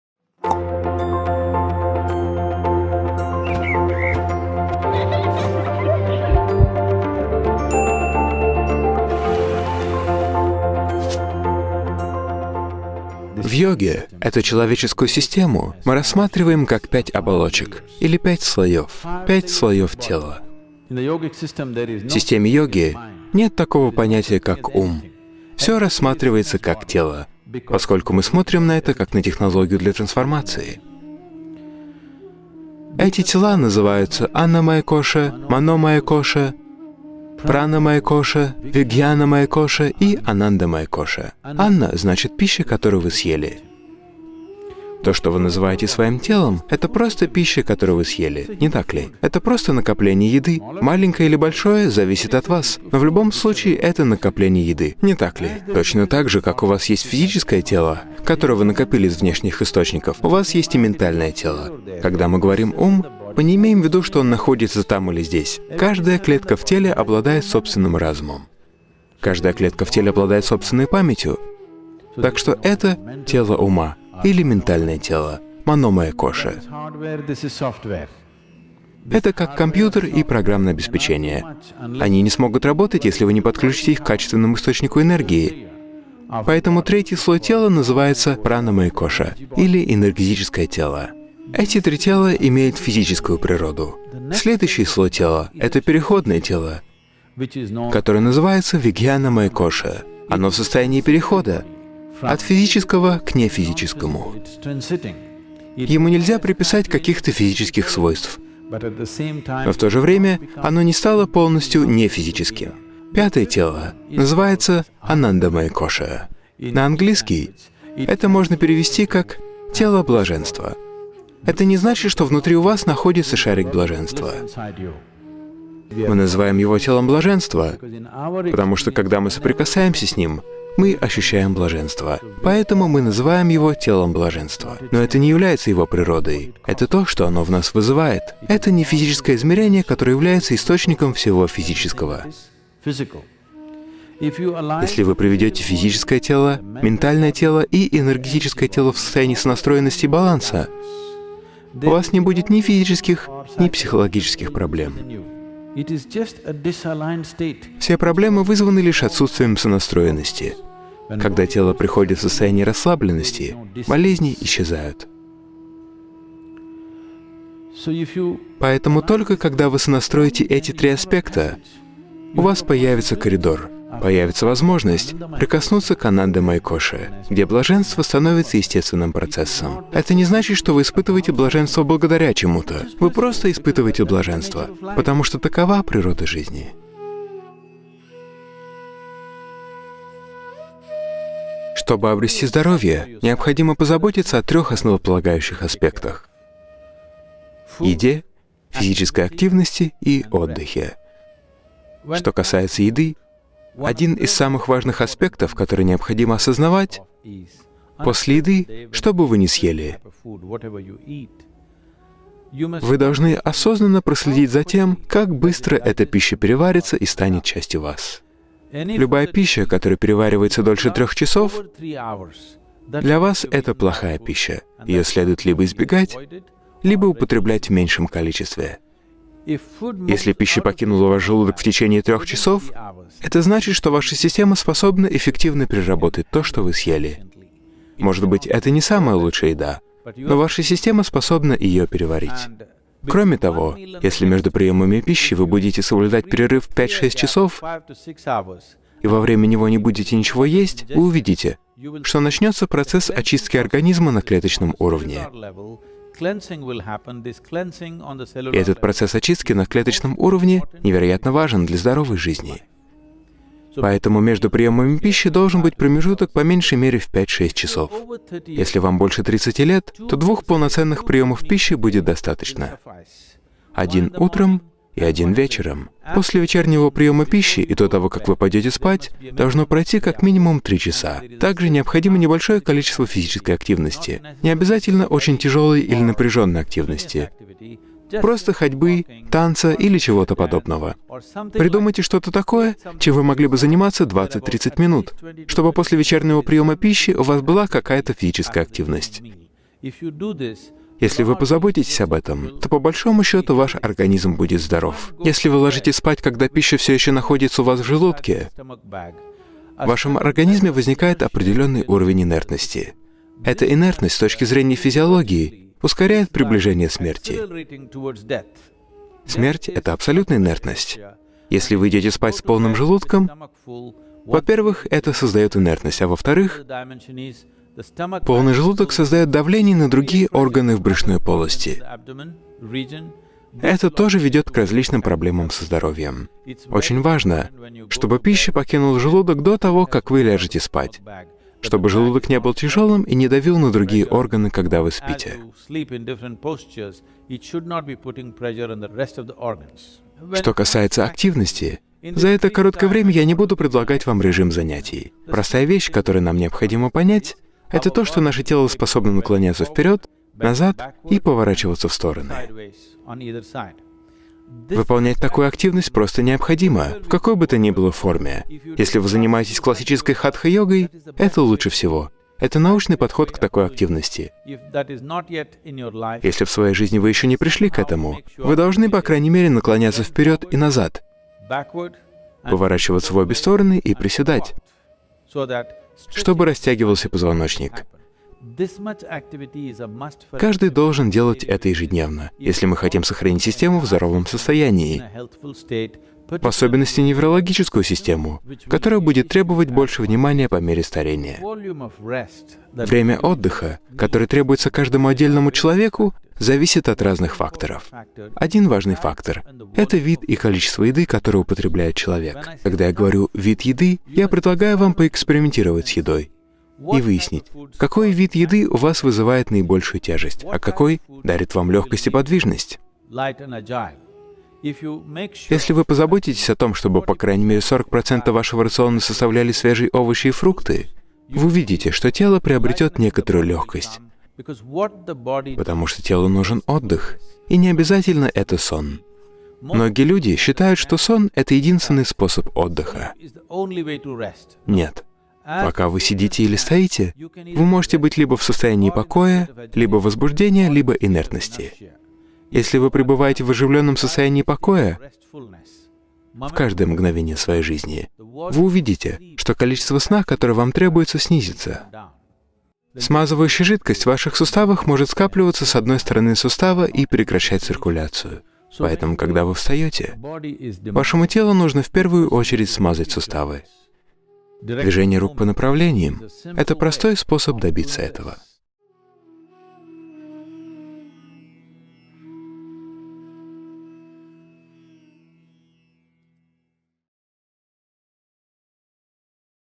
5 минут для здоровья — Садхгуру
21 июня во всем мире отмечается Международный день йоги. Отмечая важность этого дня, Садхгуру дает нам простые инструменты для здоровья, умиротворенности, исследования своей внутренней природы, радости и успеха.

В этом видео Садхгуру рассказывает о здоровье с точки зрения системы йоги, и дает несколько простых советов для обретения и поддержания здоровья.